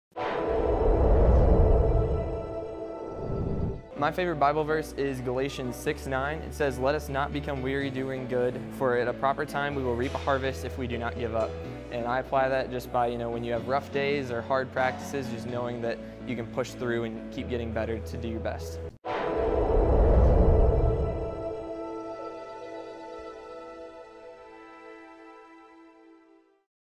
Keepers of the Flame - Josh Alexander
Rock Climbing's, Josh Alexander, shares his favorite Bible verse for Keepers of the Flame.